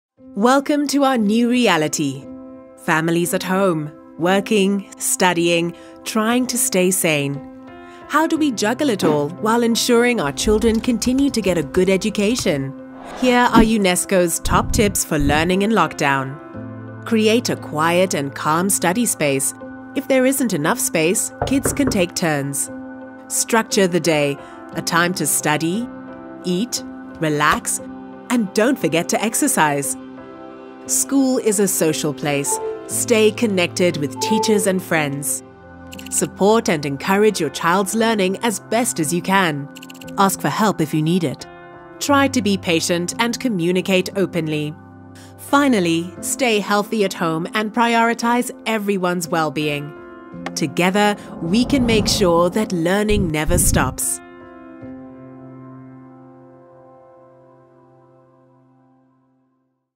Aprender durante o período de confinamento
Bem-vindos à nossa nova realidade, famílias em casa - A trabalhar, a estudar, tentando permanecer sãos. Como é que lidamos com tudo isto - Garantindo que nossos filhos continuem a ter uma boa educação? Eis as principais dicas da UNESCO para aprender durante o período de confinamento.